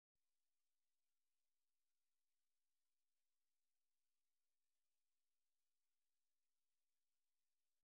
Hornets From Hell
Extreme-close-up of the textured eye of a Japanese giant hornet.